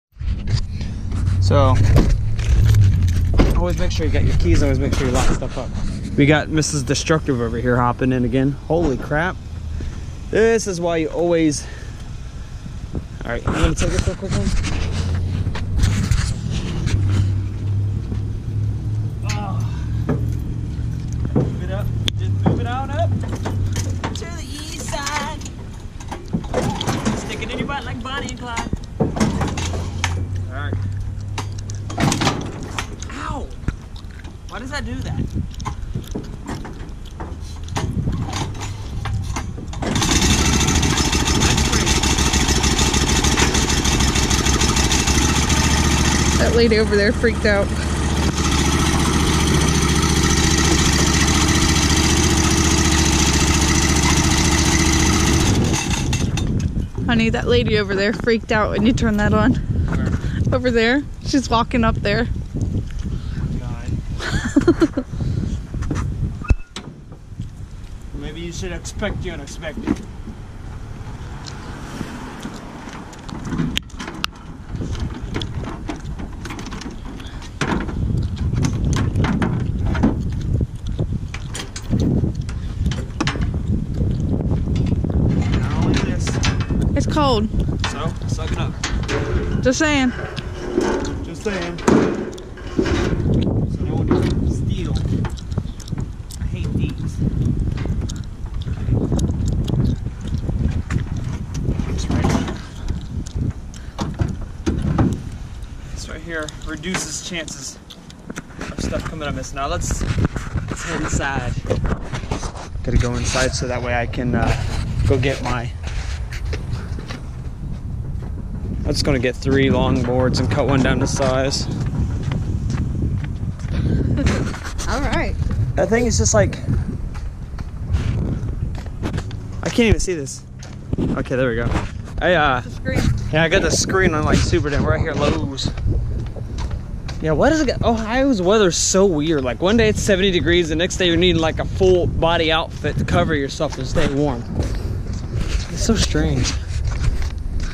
VLOG 32 SCARING PEOPLE WITH COMMERCIAL MOWERS LOL
Another daily video log of me and the wife going to Lowes to get the side wall boards for my trailer